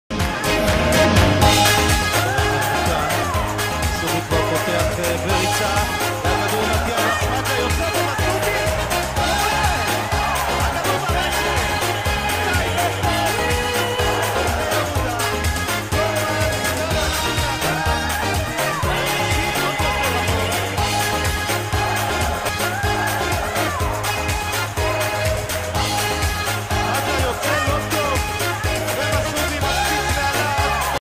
נצח מסובי
שער של החלוץ במדי בני יהודה